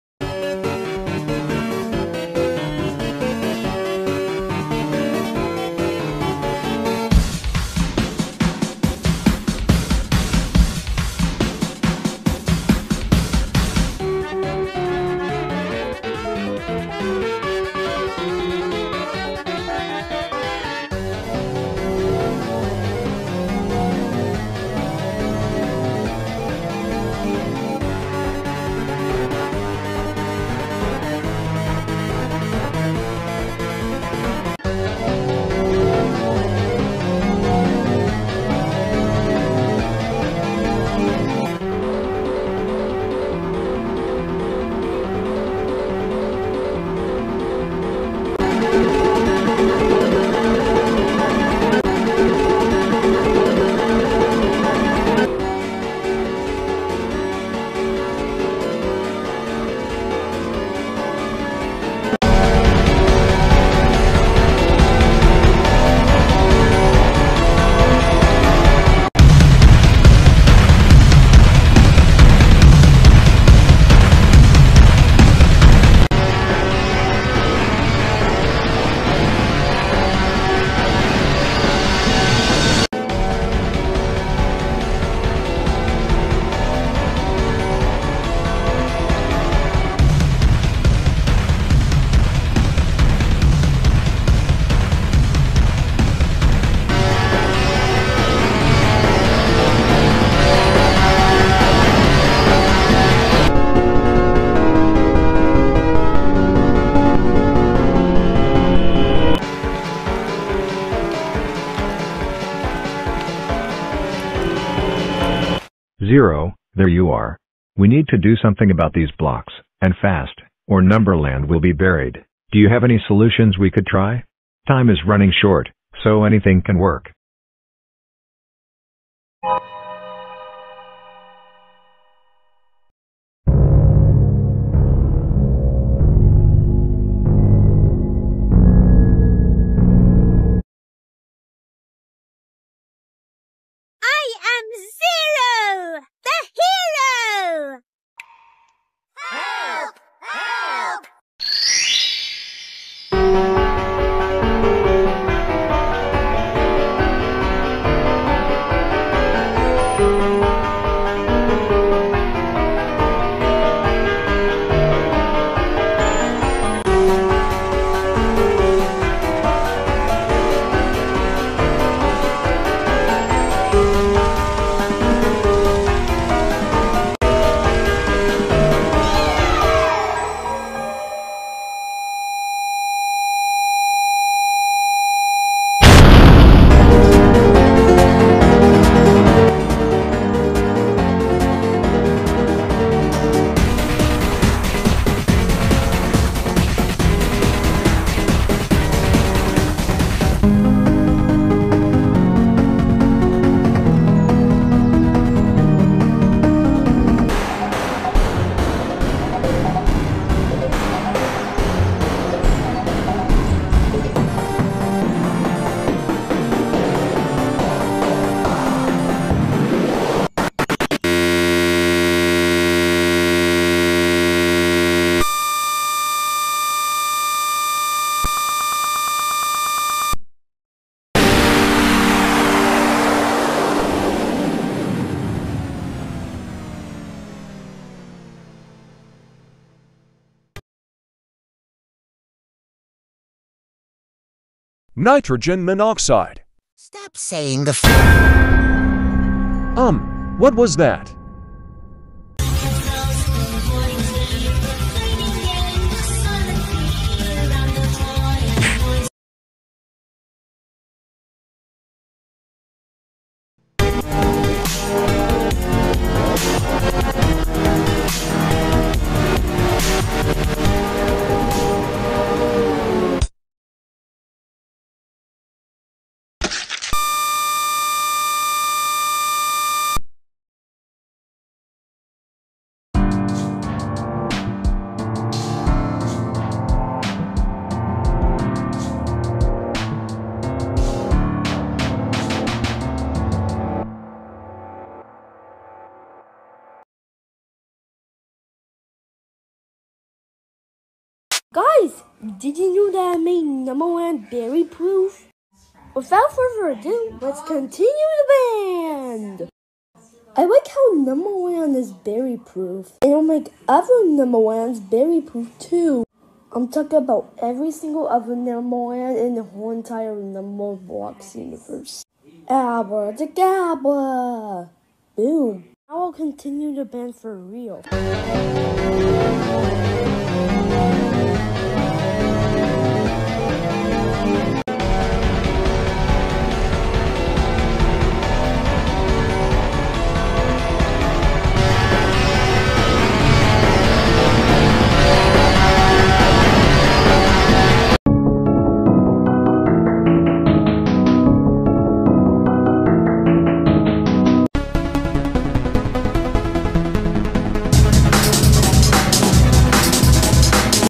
Numberblocks Integer Limits Band [Hyper-extended] (Ft. @CanadafanFantastic-x6x )
Chapters:
0:00 Intro/Small numbers
0:55 CPU-crushing numbers
1:44 Cutscene 1/5
1:56 Cutscene 2/5
2:33 Gameplay
3:06 Epilogue
3:51 Cutscene 3/5
4:24 Epilogue (Contd.)
4:43 Cutscene 4/5
5:01 Aftermath/Cutsene 5/5
5:28 The main thing (Contd.)
5:48 Outro/Hypothetical integer limits